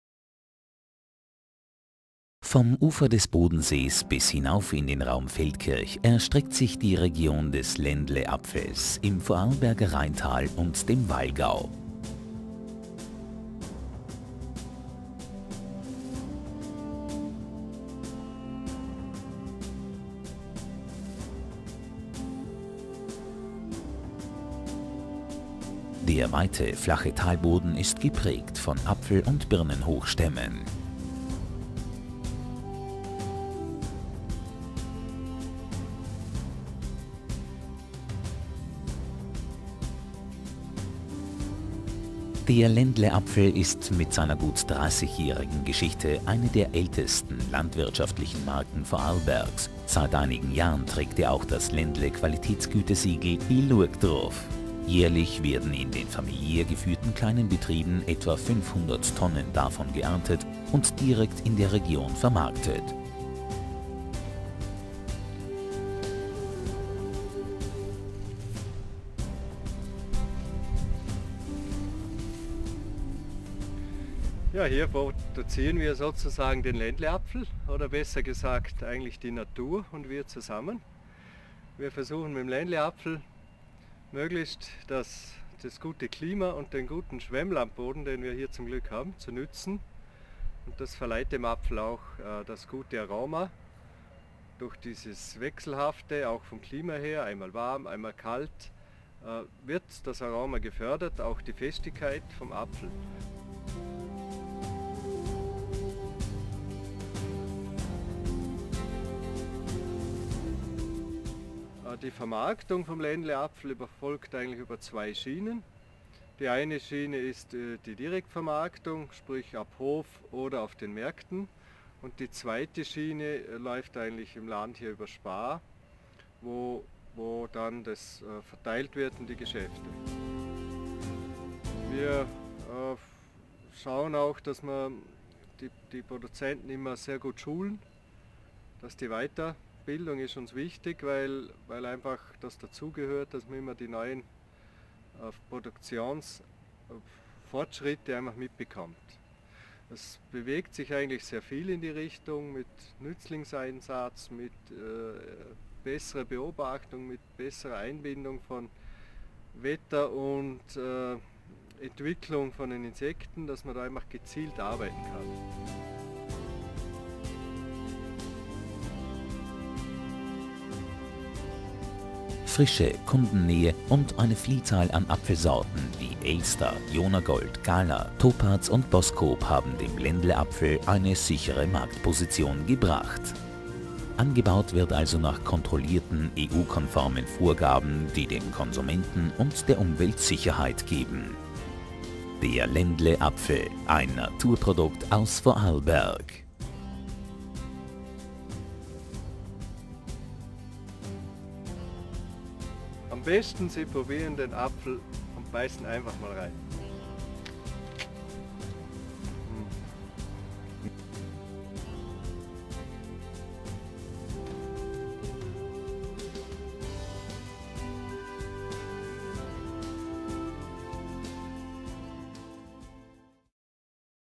Genussregion Ländle Apfel
Vom Ufer des Bodensees bis hinauf in den Raum Feldkirch erstreckt sie die Region des Ländle Apfels im Vorarlberger Rheintal und dem Walgau. Der weite, flache Talboden ist geprägt von Apfel- und Birnenhochstämmen, in den Hanglagen dominieren die Kirschen, die im Frühling mit ihrer Blütenpracht bezaubern.

Im Hintergrund erheben sich die Berge des Bregenzerwaldes, des Rätikons, des Säntismassives und des Appenzeller Landes. Beginnend mit den Freizeitmöglichkeiten des Bodensees bis hin zu den Angeboten des Hochgebirges bietet die Region immens viele Anregungen für Einheimische und Urlaubsgäste. Die mondänen Bregenzer Festspiele auf der einen Seite und die einsame Atmosphäre des Hochgebirges auf der anderen Seite spiegeln ihre Vielseitigkeit wider.

Der Ländle Apfel ist mit seiner etwa 30-jährigen Geschichte eine der ältesten landwirtschaftlichen Marken Vorarlbergs. Inzwischen trägt er auch das Ländle Gütesiegel
Jährlich werden in den familiär geführten kleinen Betrieben etwa 500 Tonnen davon geerntet und direkt in der Region vermarktet. Frische, Kundennähe und eine Vielzahl an Apfelsorten haben dem Ländle Apfel eine sichere Marktposition gebracht.
Angebaut wird nach kontrollierten, EU-konformen Vorgaben, die dem Konsumenten und er Umwelt Sicherheit geben.

Der Lebensmittelhandel weiß die Nähe der Produzenten sehr zu schätzen. Vielfach läuft der Absatz aber auch über die Direktvermarktung. Viele Produzenten bieten ihre Ländle-Äpfel ab Hof an. Ein Teil der Ländle-Äpfel werden auch veredelt zu Mosten, Fruchtsäften und Edelbränden.

Die Region zwischen Bodensee und Walgau eignet sich hervorragend für den Apfelanbau. Tiefgründige, nahrhafte Boden, gemäßigte Temperaturen sowohl im Winter als auch im Sommer und ausreichende Niederschläge bieten dem Apfel ideale Wachstumsbedingungen. Stabiles, trockenes Herbstwetter mit bereits niedrigen Nachttemperaturen geben dem Ländle Apfel vor der Ernte noch eine gute Portion an Aromastoffen und Farbe.